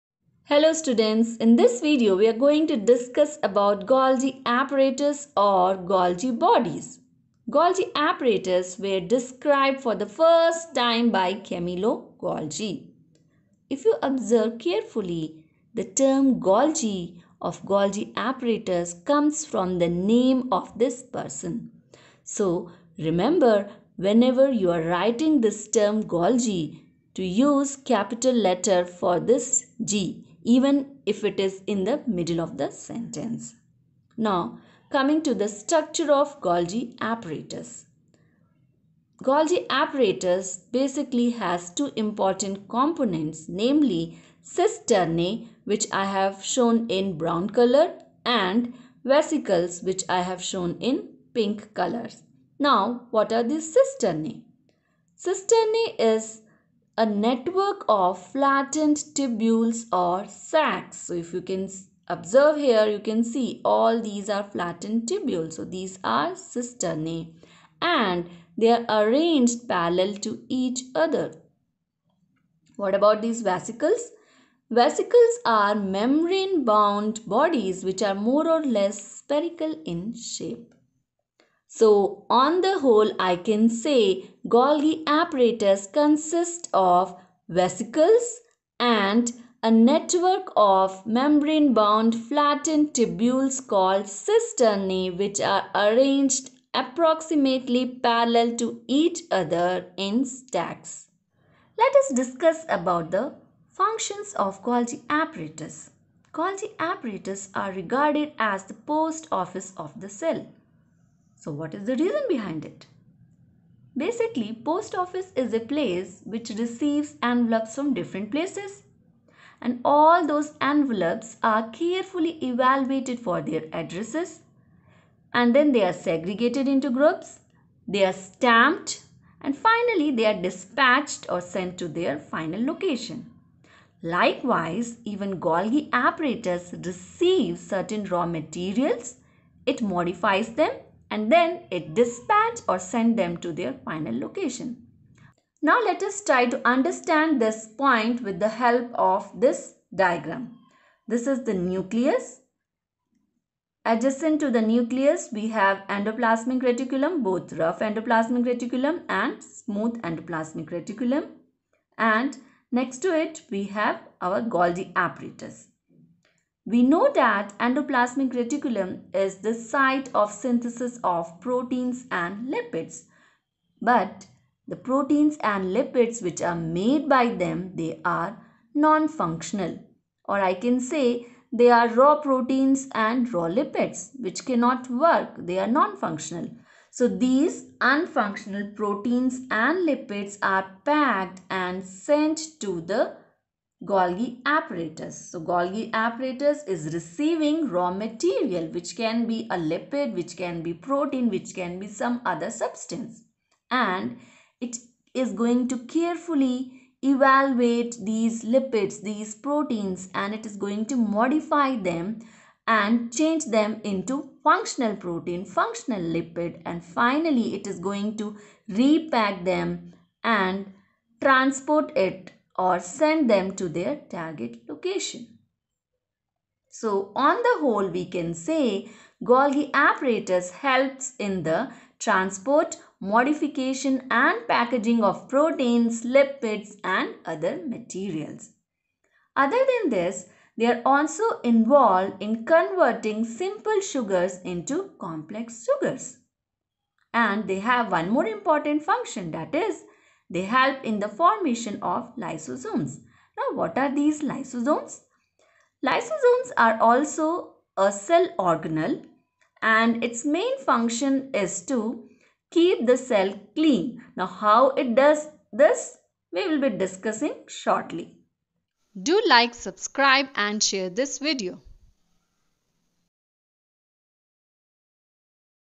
Function of Golgi Apparatus Class 9|Biology|CBSE
Hi friends,
         In this video we will learn about the Golgi Apparatus Structure  and Function.